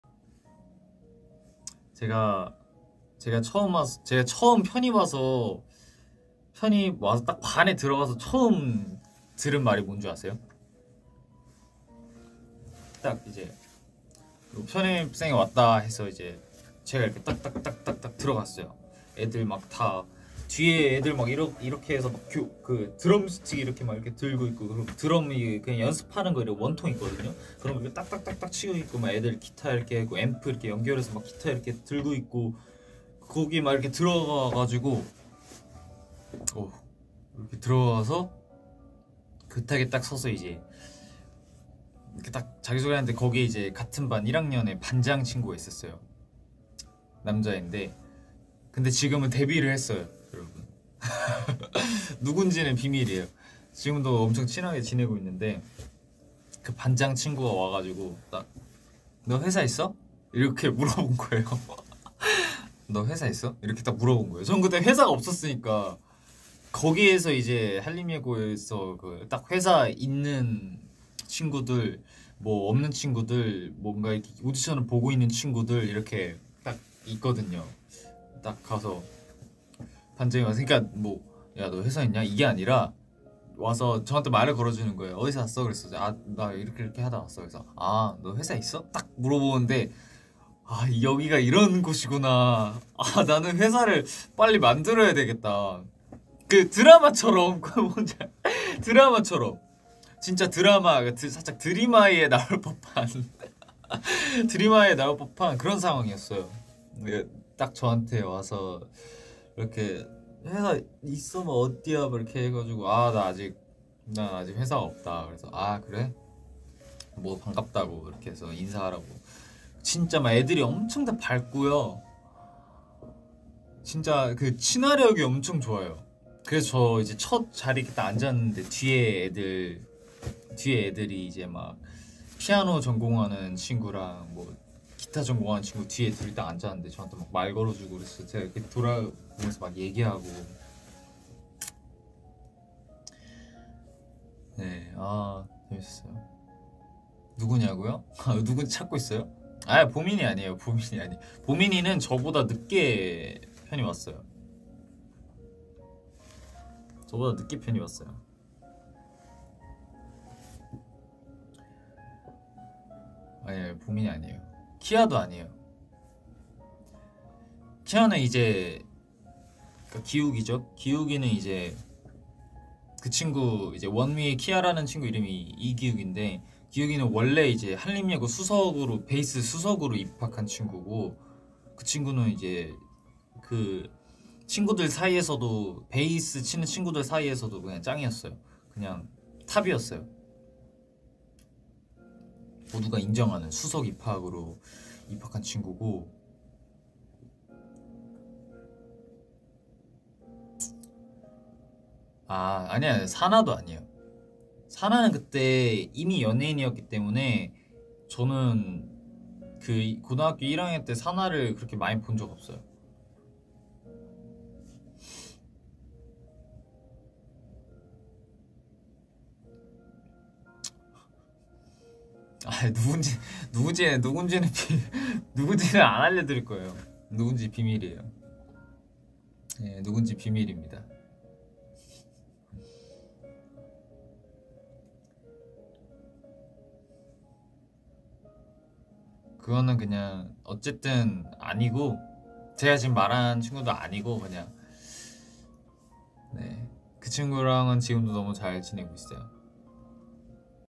[ENG] TBZ - Sunwoo’s first day at Hanlim (feat. Cya)
subbed by @leedoeus
translation checked by @bossbabyjkh